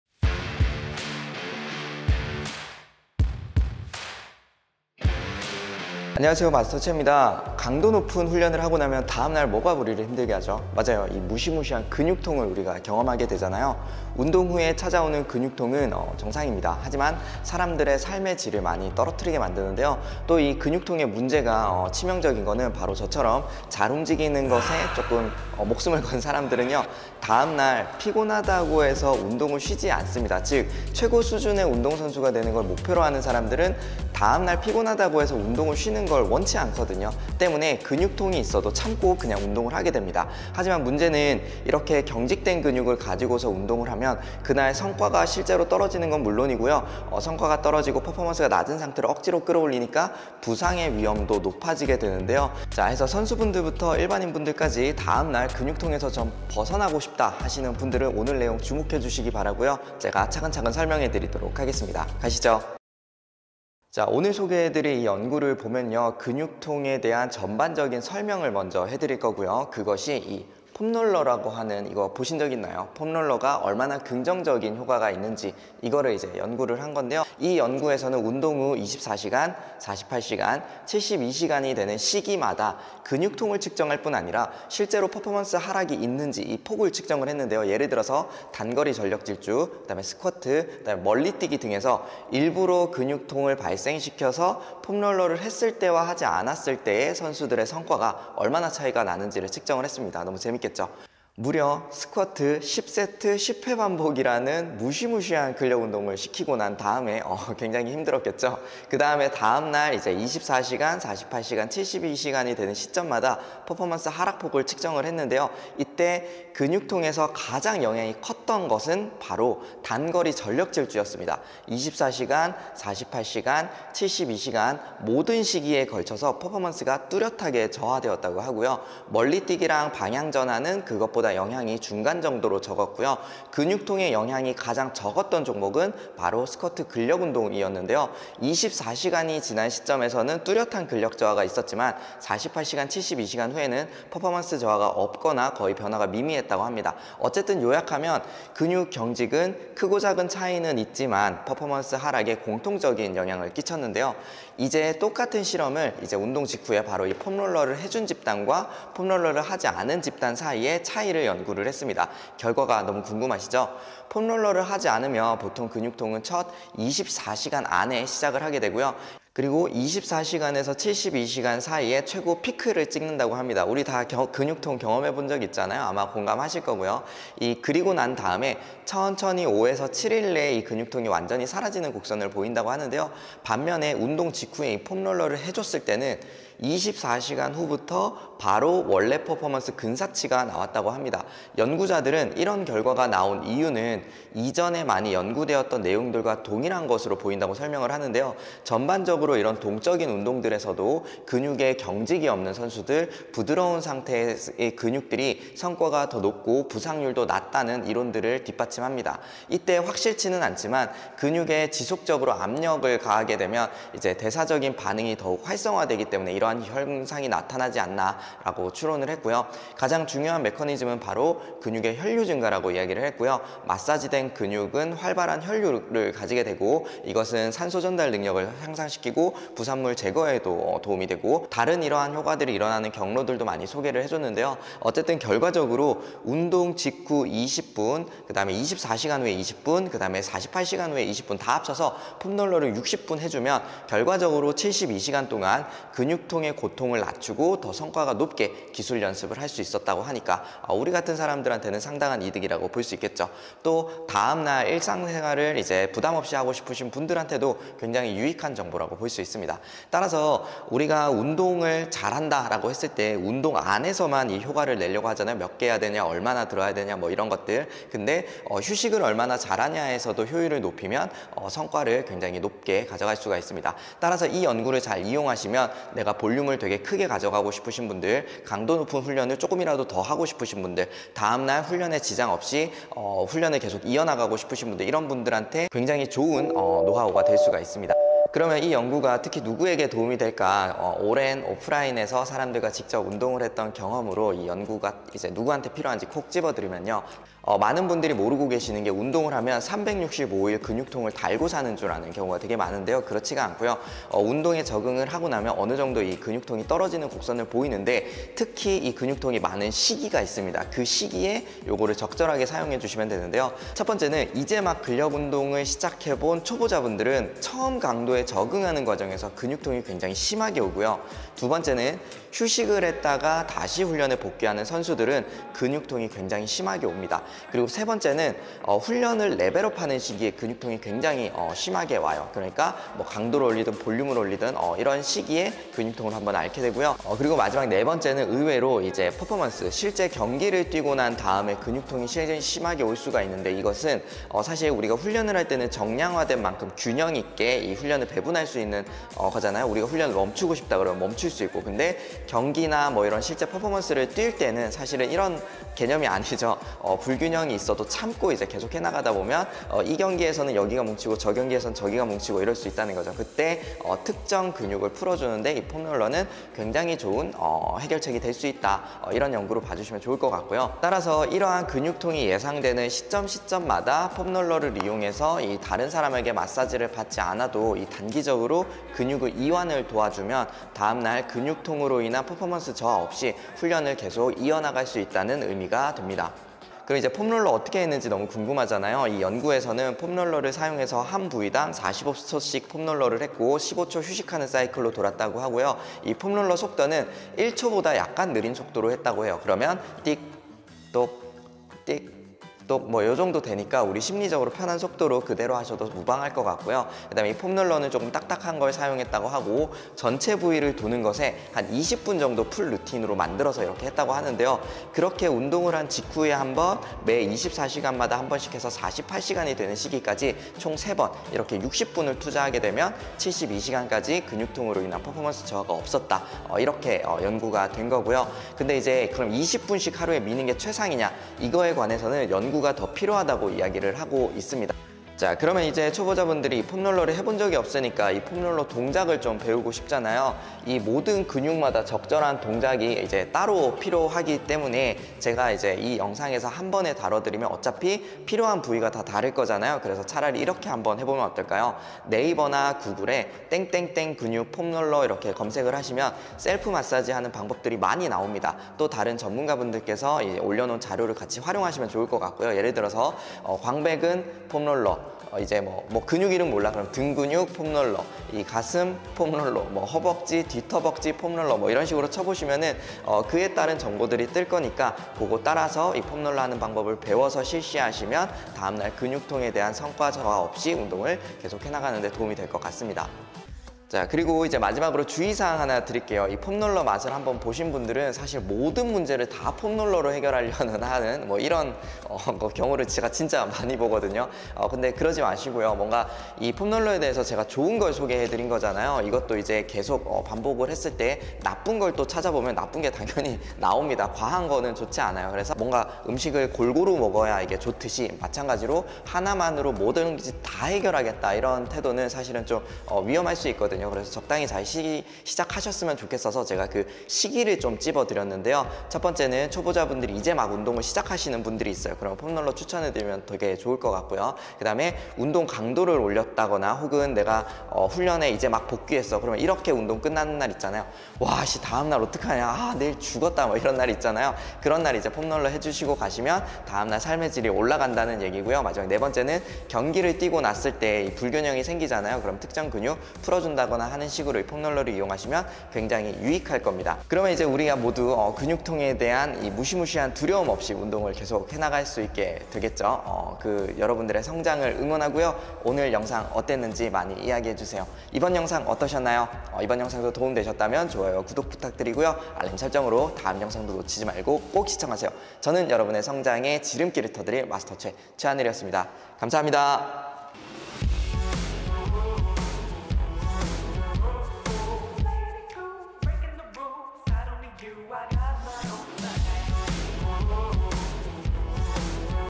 SUB) 근육통 빨리 푸는 법 드디어 밝혀져!! 👍 | 과학적 근거 & 운동 후 통증 걱정 끝!!
*[ 🏛️ 움직이며 사는 삶 / 싱무브 그라운드 도곡동 교육센터 ]

근육통의 걱정 없이 훈련 볼륨이나 강도를 다른 사람들보다 더 크게 가져가고 싶은 분들에게 크게 도움이 될 것 입니다. 초보자 분들에게도 마찬가지고요!

INTRO
00:00 무엇을 배우는가?
01:03 근육통과 퍼포먼스 저하
02:34 폼롤러가 근육통에 미치는 영향
04:50 폼 롤러 누구에게, 언제 도움이 될까?
06:27 폼 롤러 어떻게 하죠?
08:00 폼 롤러 만병통치약이 아닙니다.

마스터 최 채널은 영상 퀄리티를 높이기 위해 자유연재로 업로드가 됩니다. ^^

*주의사항! : 이 영상은 정보제공용으로 제작된 것으로서 의학적 조언이 아닙니다. 필요시 의사와 같은 믿을 만한 전문가에게 꼭 도움을 구합시다.